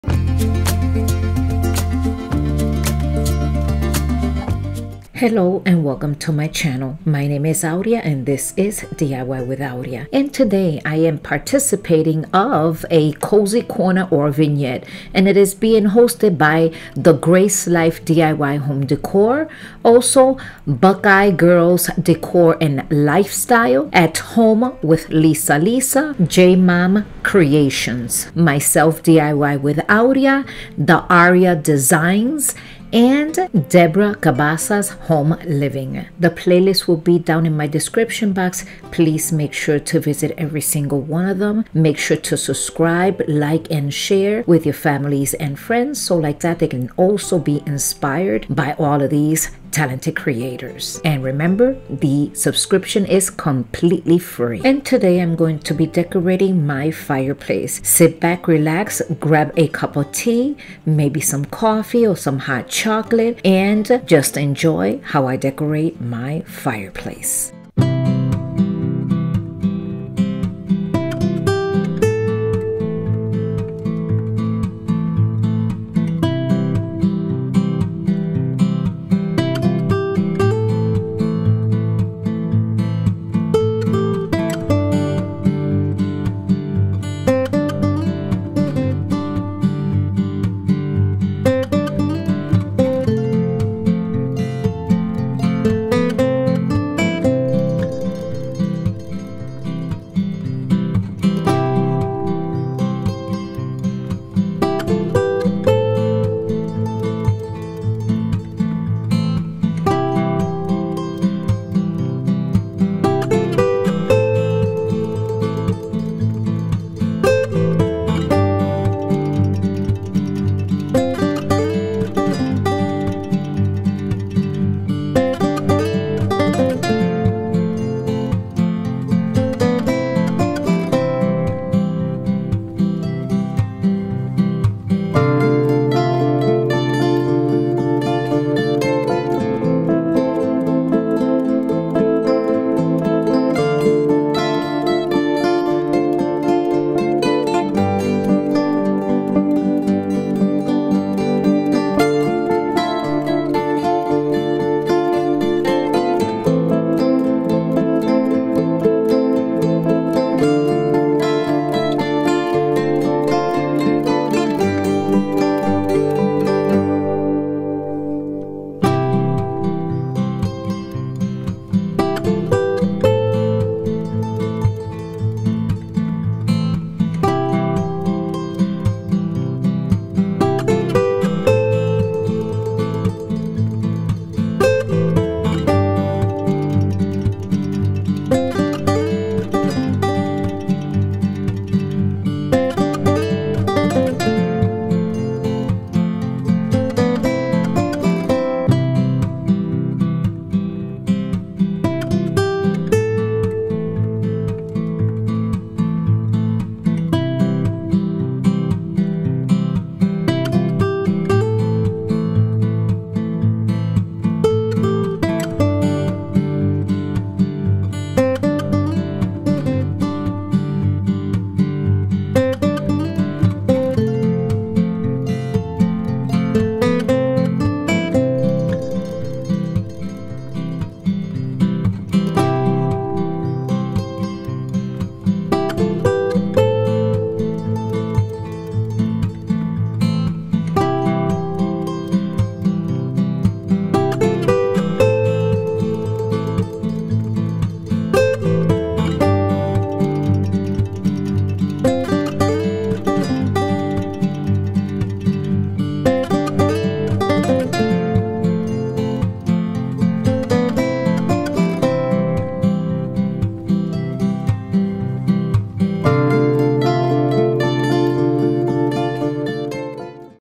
"Captivating Cozy Corner Collab - Don't Miss Out!"
Hello my dear friends, today I am participating in an open challenge and the name is cozy corner or vignette. It is being hosted by the Grace Life DIY home decor and buckeye girls decor and lifestyle. also at home with Lisa, Lisa and Jmom creations The Aria designz and also Deborah Cabassa home living. Make sure to go below and hit that playlist so you can see all of these wonderful talented creators. show you some more inspiration!